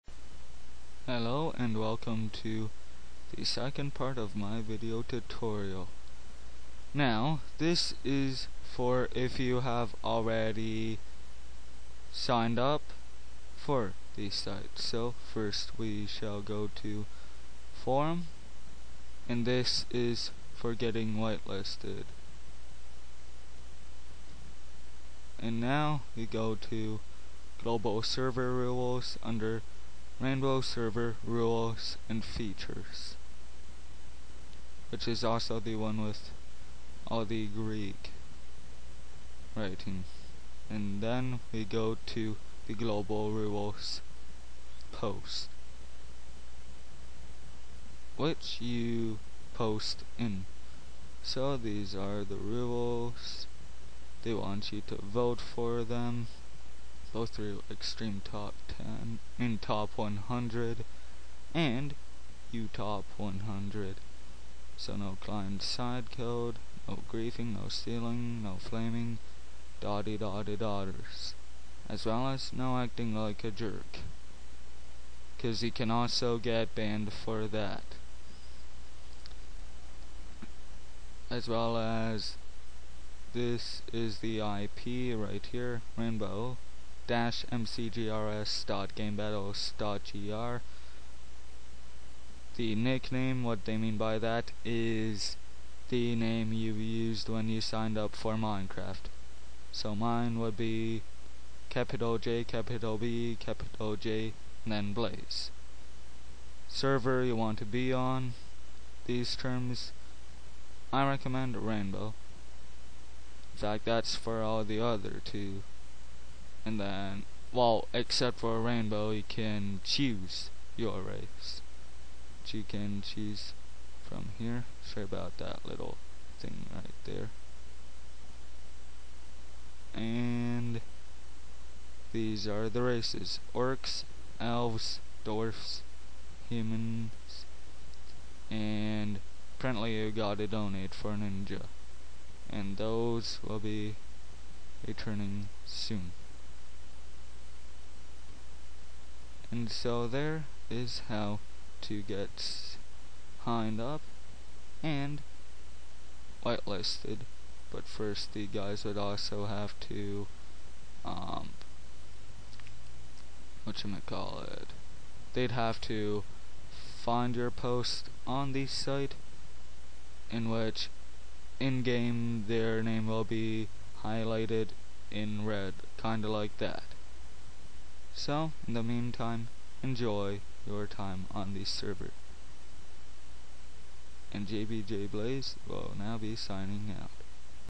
How To Sign Up and Get White-listed on Rainbow Server - Part 2: How to Get White-listed
Many new players - or 'villager's - come onto this server, and for some time, they'll just leave getting White-listed alone while they are on. This Video Tutorial is to help those confused people become less confused, sign up, and get White-listed on Rainbow Server.